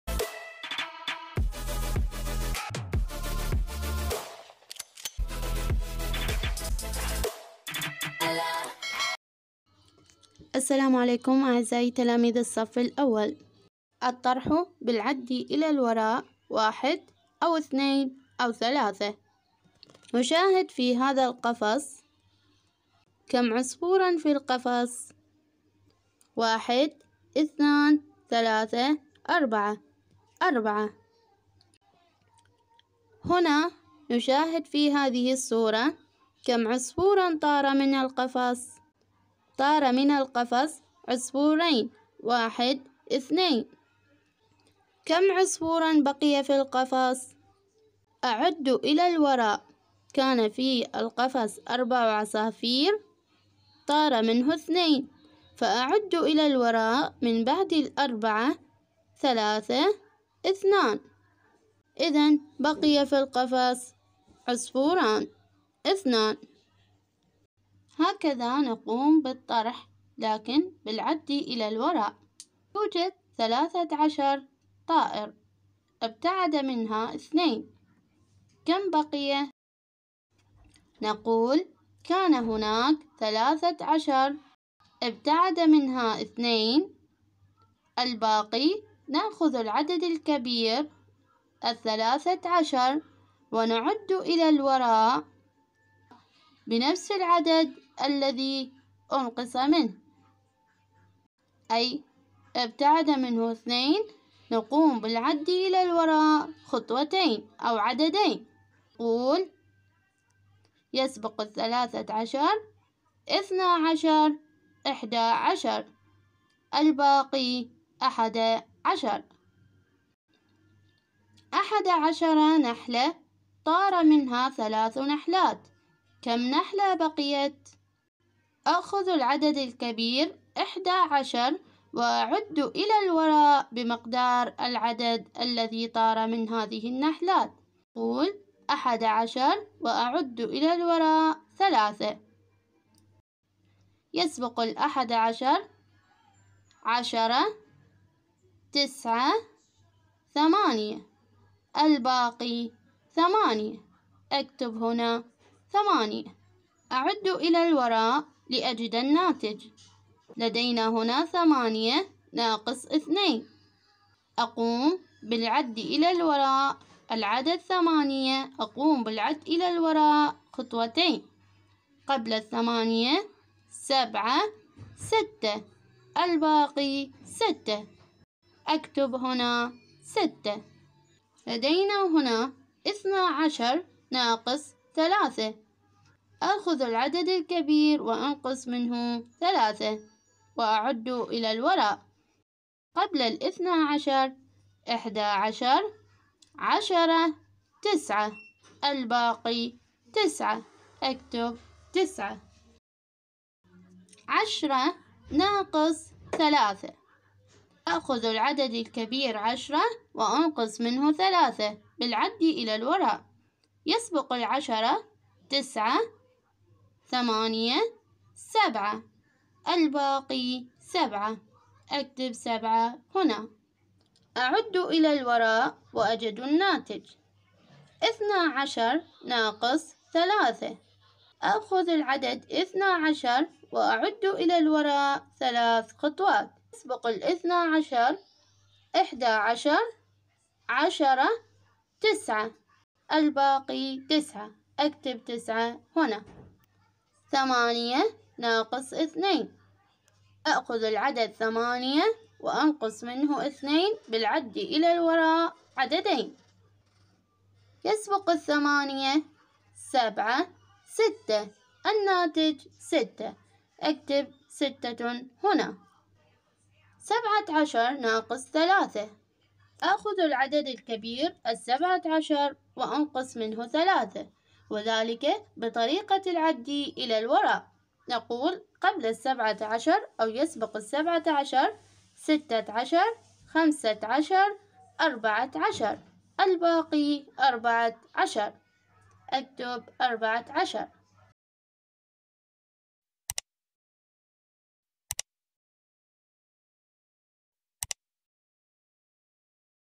الطرح بالعد الى الوراء ١أو ٢ أو ٣ رياضيات الاول الابتدائي
رياضيات الاول الابتدائي
الطرح مفهوم الطرح الناقص
الطرح بالعد إلى الوراء ١أو ٢أو ٣
ابسط الطرق للتعلم معنا
مع تحيات كادر قناة نوافذ التعلم